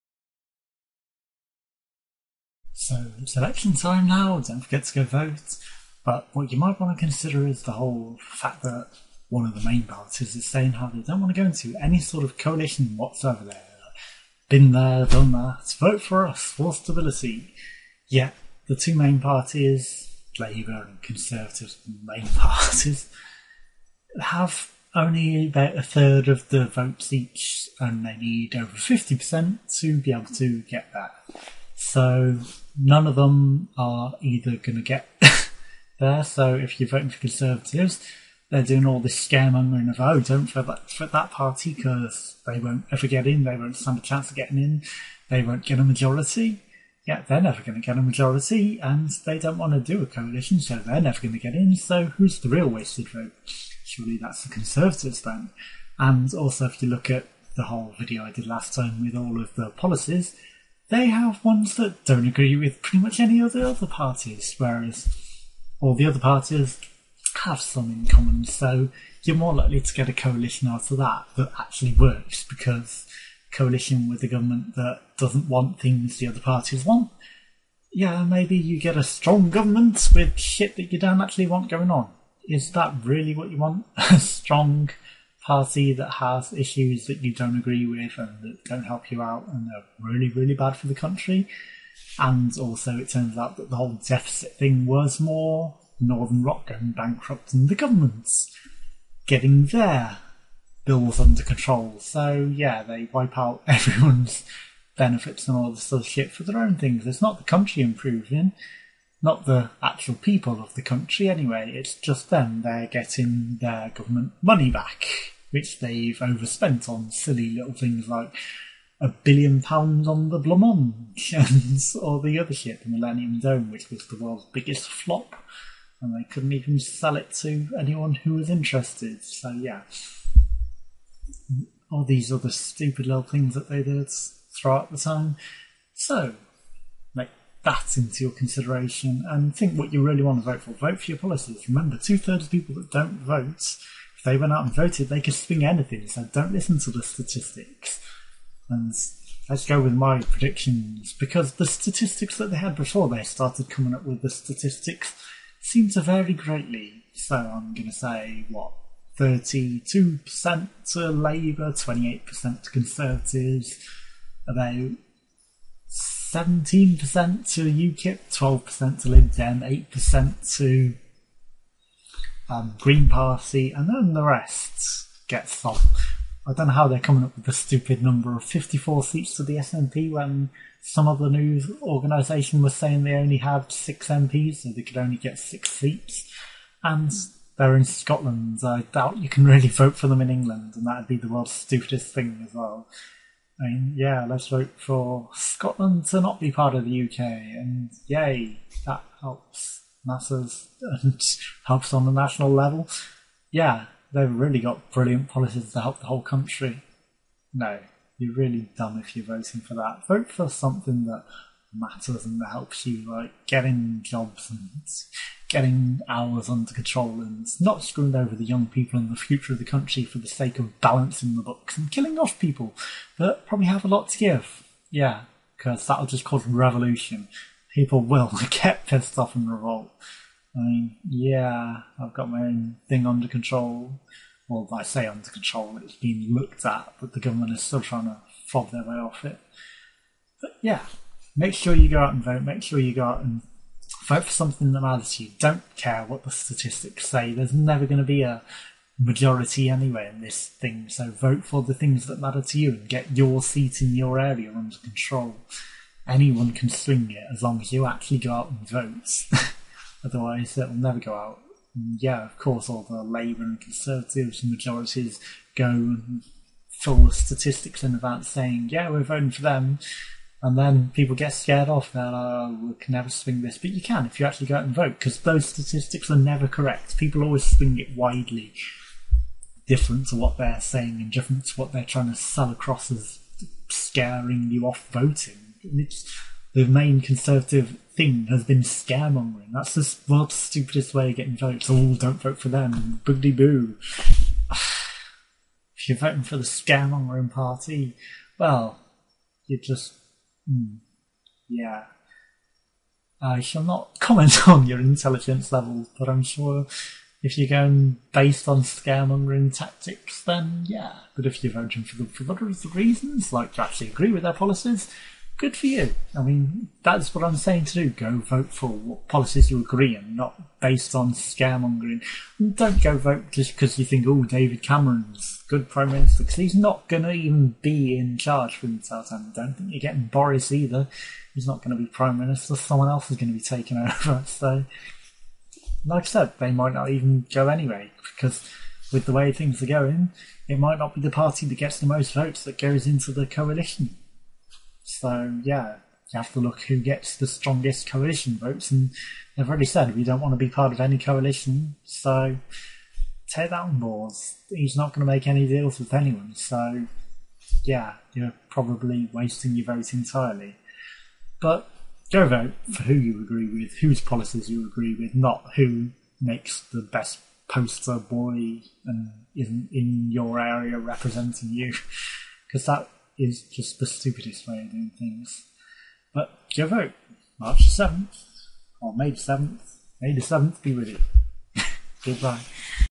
Vote On May 7TH In The UK On Policies That Matter To You. + Predictions
Remember to go vote on may 7th, and vote on policies that matter to you, not based on spin, or based around "predicted outcomes" where the statistics never match up to the actual fact anyway, remember you can make a difference, and voting on policies that really matter rather than strength in policies which are bad for you and the country, and voting for a party who doesn't want a coalition where no party stands a chance of getting a majority would be a bad idea, but thats your decision, as long as you're making an informed choice rather than being influenced by silly things.  And don't forget david cameron won't even be a full term prime minister.